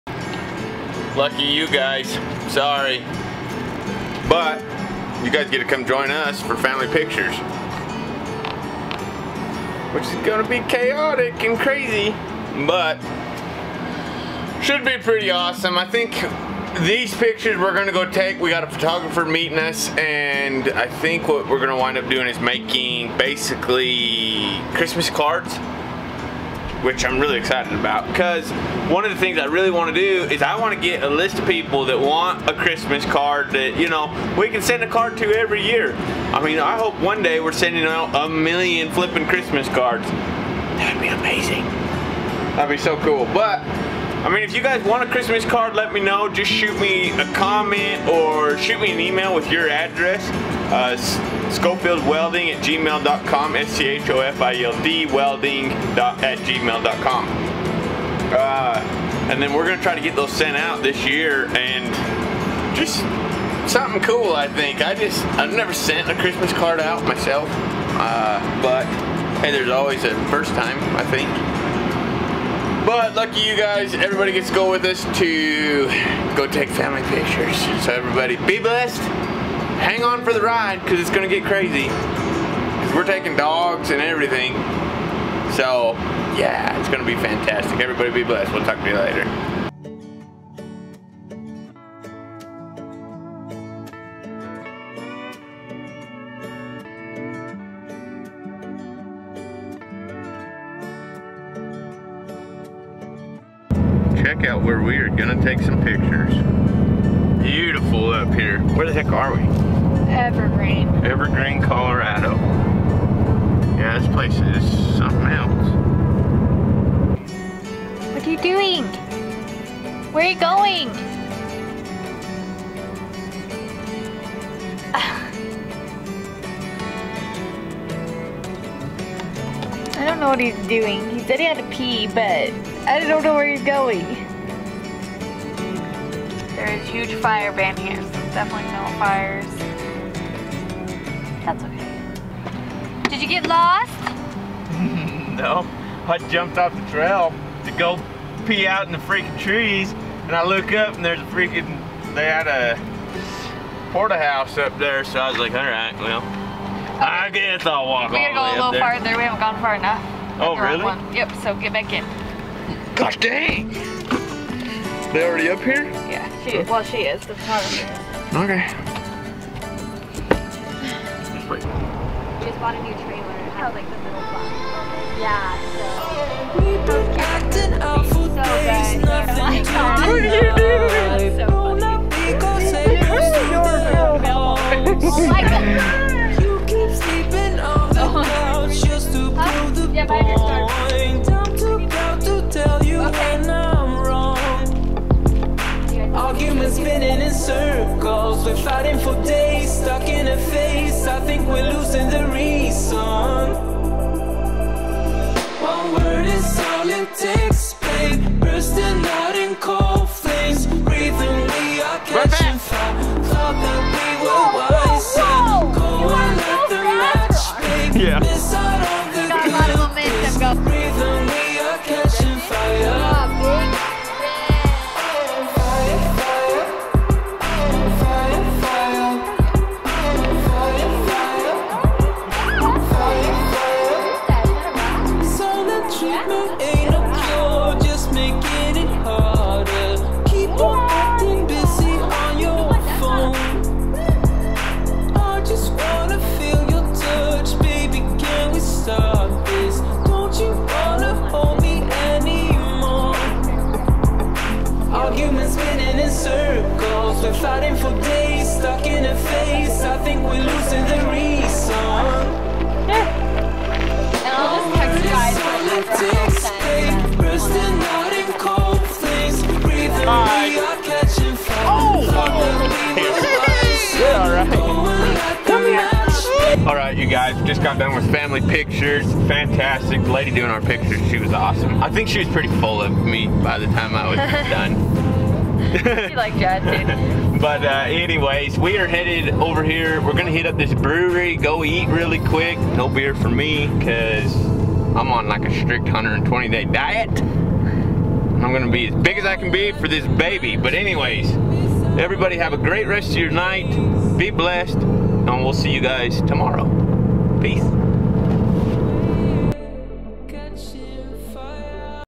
Schofield Welding We bring The chaos Family Pictures
Great time getting pictures taken with Josh and Haley with there little family. Great great family and we are blessed to have met them. Everyone be blessed have a great weekend and get loaded down because we have money to chase come Monday Morning.